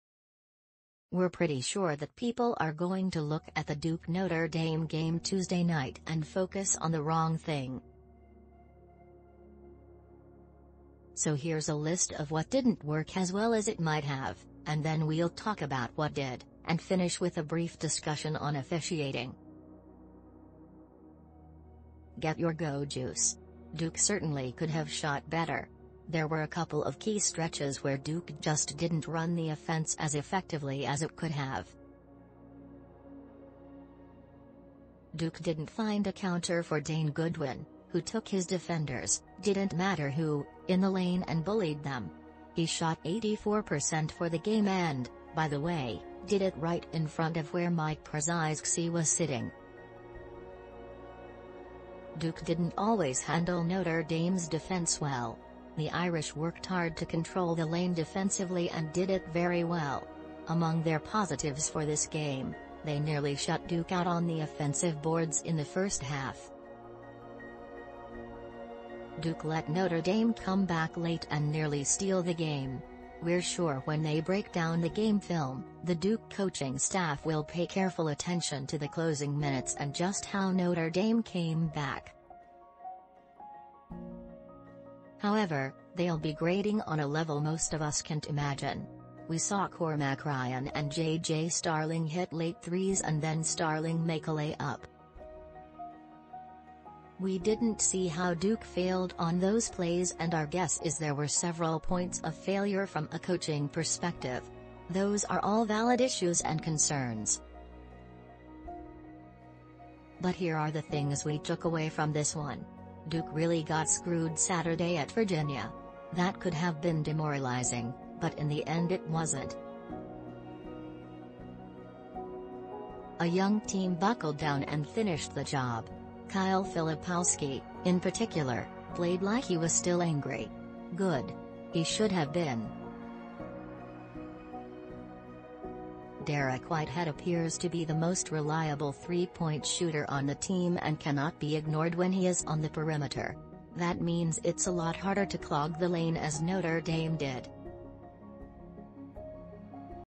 More On Duke’s Win Over Notre Dame The Team’s Progress And How The ACC
We’re pretty sure that people are going to look at the Duke-Notre Dame game Tuesday night and focus on the wrong thing. So here’s a list of what didn’t work as well as it might have, and then we’ll talk about what did, and finish with a brief discussion on officiating. Get your go juice! Duke certainly could have shot better. There were a couple of key stretches where Duke just didn’t run the offense as effectively as it could have. Duke didn’t find a counter for Dane Goodwin, who took his defenders - didn't matter who - in the lane and bullied them. He shot 84 percent for the game and, by the way, did it right in front of where Mike Krzyzewksi was sitting. Duke didn’t always handle Notre Dame’s defense well. The Irish worked hard to control the lane defensively and did it very well. Among their positives for this game, they nearly shut Duke out on the offensive boards in the first half. Duke let Notre Dame come back late and nearly steal the game. We’re sure when they break down the game film, the Duke coaching staff will pay careful attention to the closing minutes and just how Notre Dame came back. However, they’ll be grading on a level most of us can’t imagine. We saw Cormac Ryan and JJ Starling hit late threes and then Starling make a layup. We didn’t see how Duke failed on those plays and our guess is there were several points of failure from a coaching perspective. Those are all valid issues and concerns. But here are the things we took away from this one. Duke really got screwed Saturday at Virginia. That could have been demoralizing, but in the end it wasn’t. A young team buckled down and finished the job. Kyle Filipowski, in particular, played like he was still angry. Good. He should have been. Dariq Whitehead appears to be the most reliable three point shooter on the team and cannot be ignored when he is on the perimeter. That means it’s a lot harder to clog the lane as Notre Dame did. That’s potentially huge for Duke. If you haven’t realized it yet, Scheyer is looking for shooters. He signed Jaden Schutt, who is developing behind the scenes, and also has a commitment from TJ Powers in 2023 and Darren Harris in 2024. This year Duke has had a tough time on the three. That’s not going to be the case for long. Over the course of this season, we’ve seen three major problems for Duke, all related to youth: turnovers, offensive stalls and uncertainty. And really, you could probably reduce that to just the latter. For instance, at Miami, Tyrese Proctor was very tentative. Not Tuesday night. He took the ball to the basket with 4:15 left for a key basket and ran the team very well. Duke had eight turnovers Tuesday night and the last one came with 7:18 to play. We had a lot of concern about Notre Dame’s experience and it came through for them with that very fast comeback at the end, but here’s perhaps the main takeaway: Duke didn’t break.